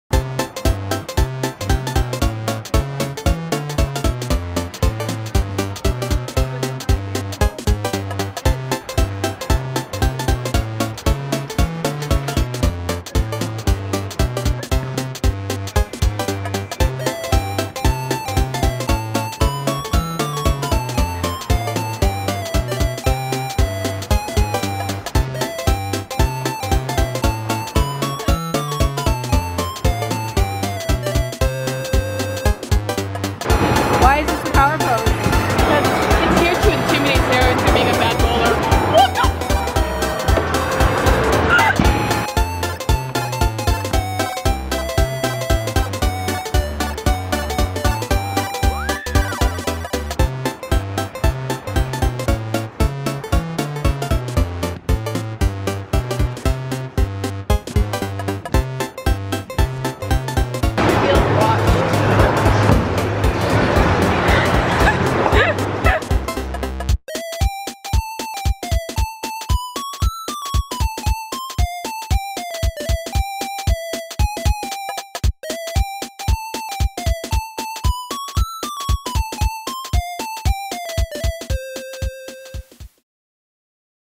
rented shoes & neon lights | cosmic bowling vlog
22.06.17 | "Sarah, we're gonna need you to be less good at this"
In which Sarah, Sharmaine, Kaity & I go bowling.